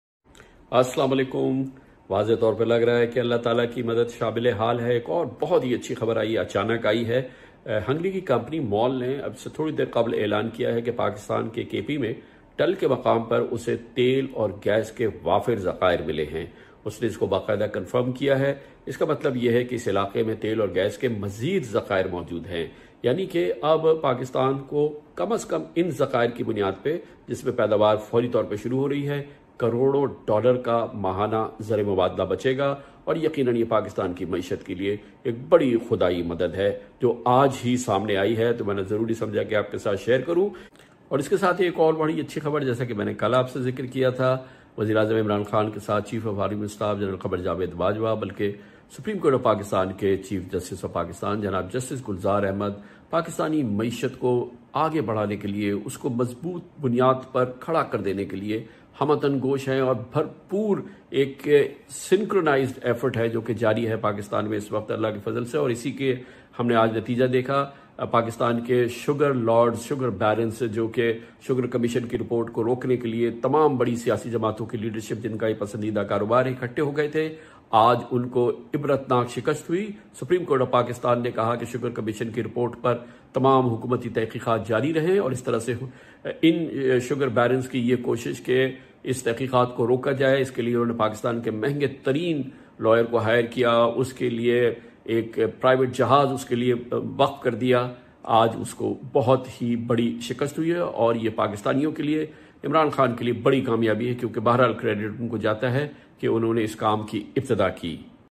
||The good news |is that oil and| gas reserves| have been| discovered| at the KP oil site.| Hangu's
||The good news |is that oil and| gas reserves| have been| discovered| at the KP oil site.| Hangu's |receiving committee| declares| that thousands of| barrels| of oil will be |available| daily.|